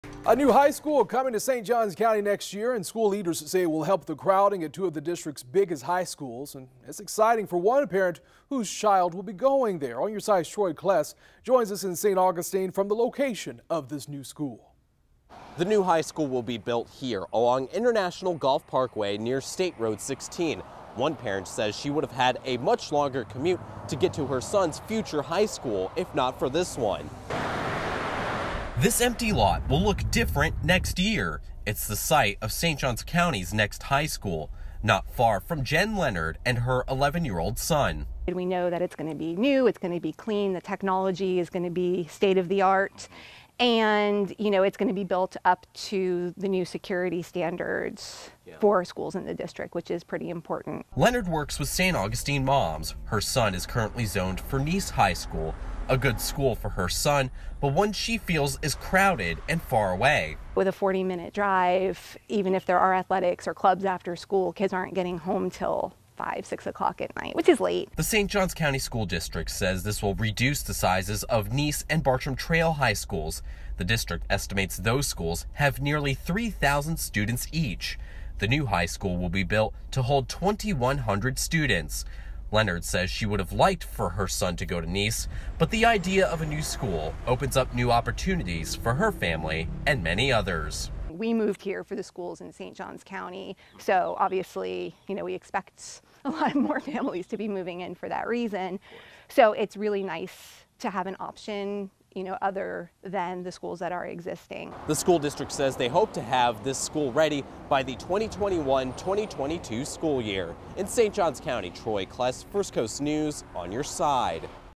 New St. Johns County School being built closer to home for some First Coast families
One parent says the new high school, expected to be finished in 2021, will cut down what could have been a 30 to 40 minute commute to Nease High School.